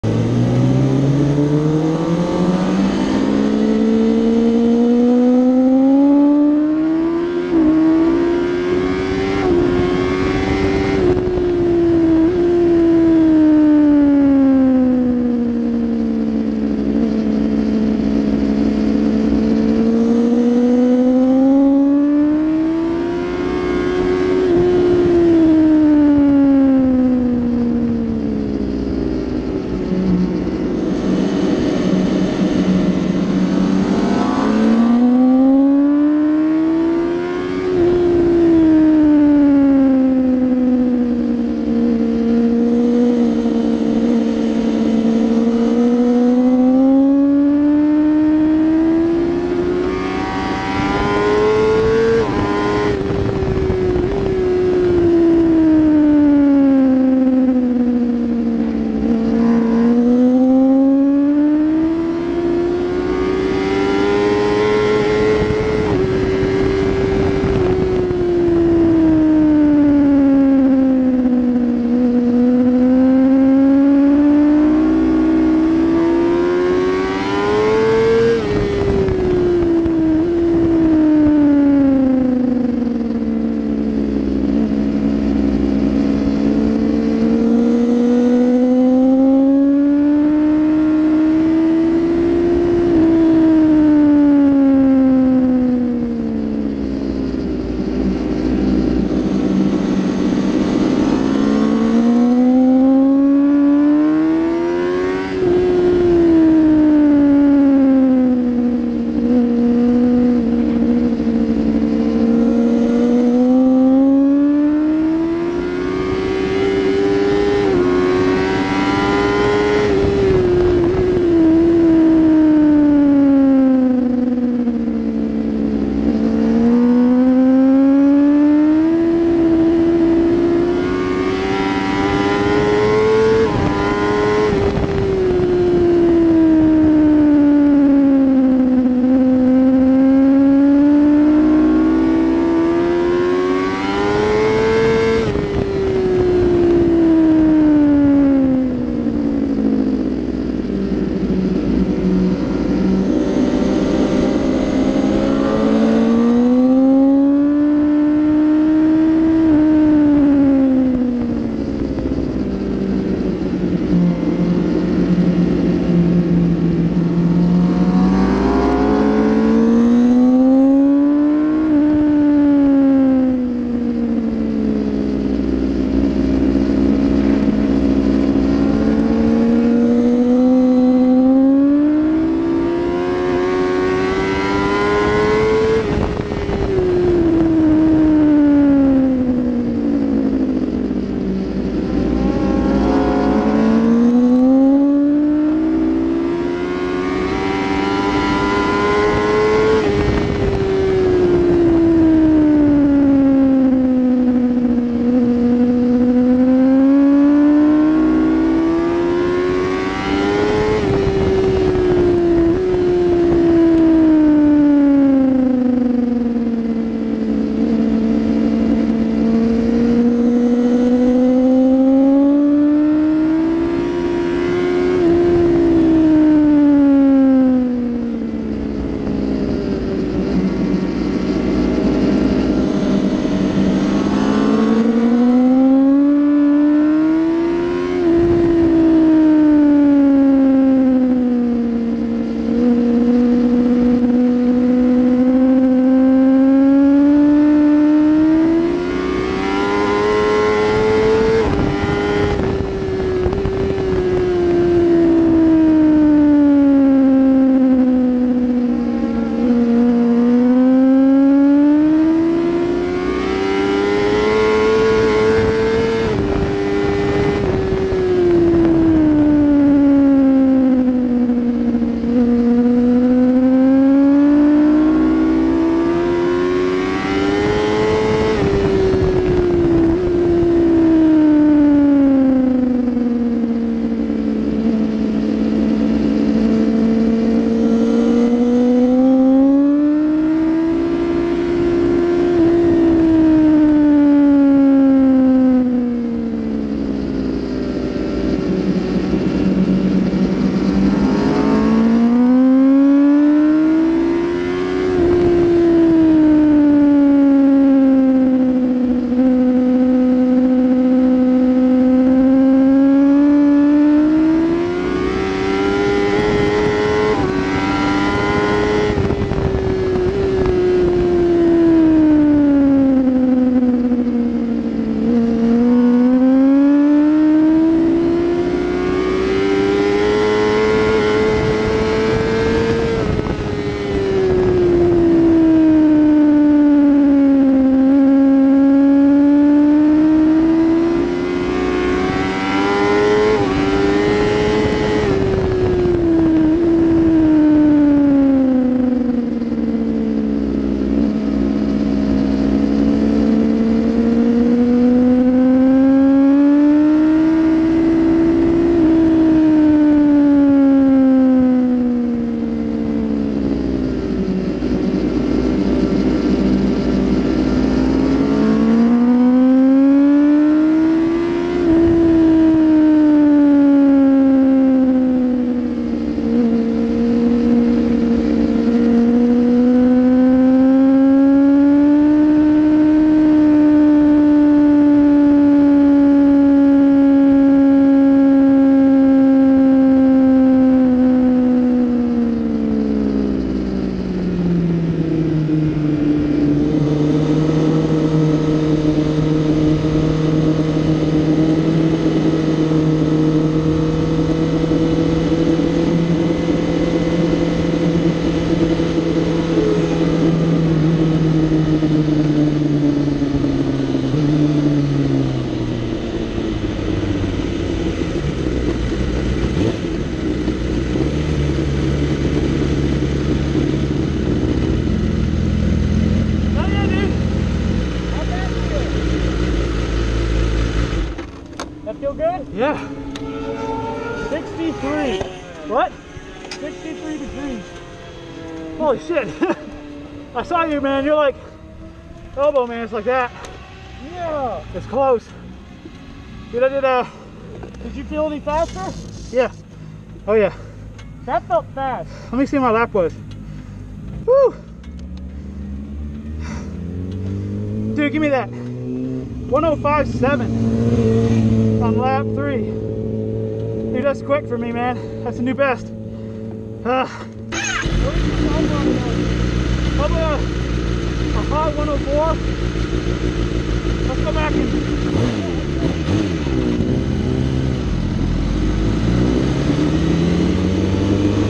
ZX10R VS S1KRR TRACKDAY: buddy on a 2018 BMW S1000RR while I chase on 2016 ZX10R!
Sportbiketracktime.com/Advance Group/June 22 2019
2016 ZX10R following a buddy on a 2018 BMW S1000RR.  Dash is an AiM MXK10.

Shot with a GoPro Hero 4 Session mounted on the upper triple clamp.